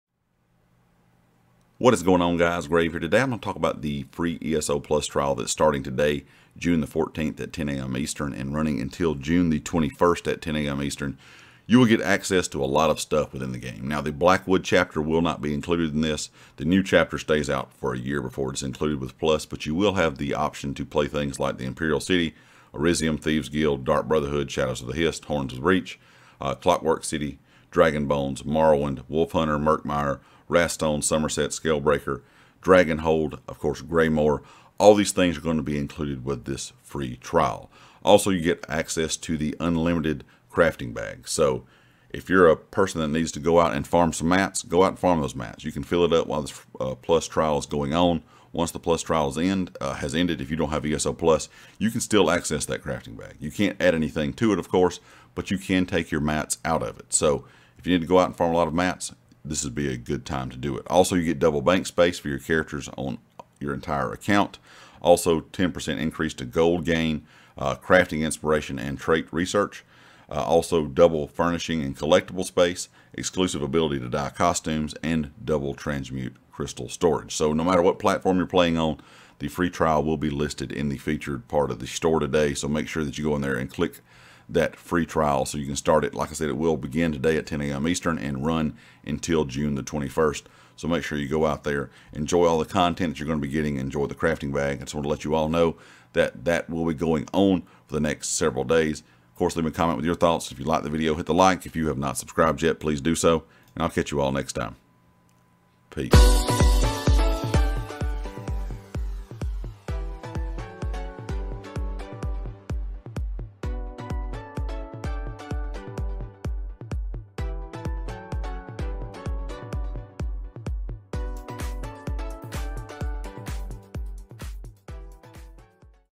🔍Free ESO Plus Trail Starts Today! | Elder Scrolls Online
ESO plus will be free starting June 14th till June 21st.